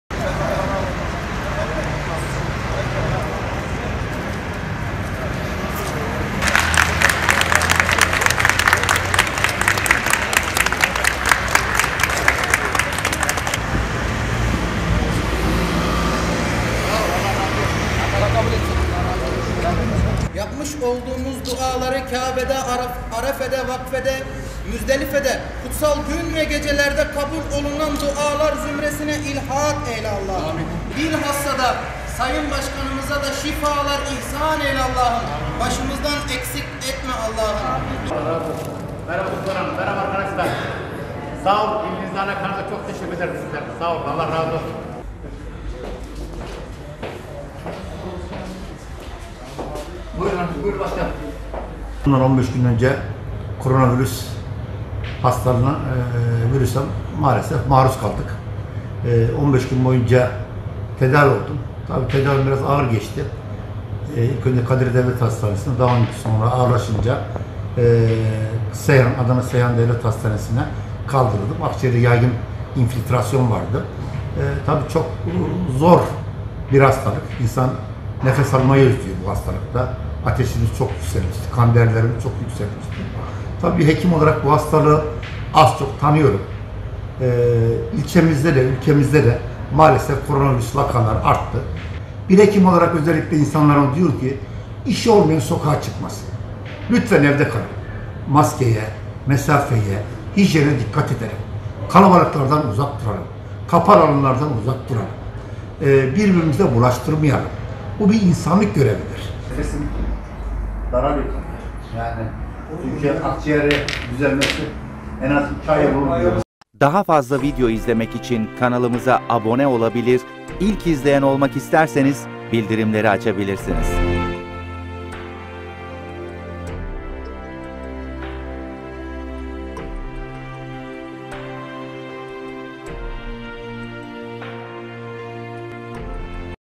Kovid-19'u yenen Kadirli Belediye Başkanı Tarhan yeniden görevine başladı | Son Dakika Haber
Yeni tip koronavirüs (Kovid-19) tedavisi ve karantina süreci tamamlanan Kadirli Belediye Başkanı Ömer Tarhan, görevine başladı.
Belediye personelinin bina önünde ellerinde çiçeklerle, kurban keserek karşıladığı Tarhan, daha sonra makamına geçti.

Tarhan, AA muhabirine yaptığı açıklamada, Kovid-19 kaynaklı rahatsızlığı sebebiyle 15 gün süren tedavisinin çok zor geçtiğini söyledi. | Son Dakika Haber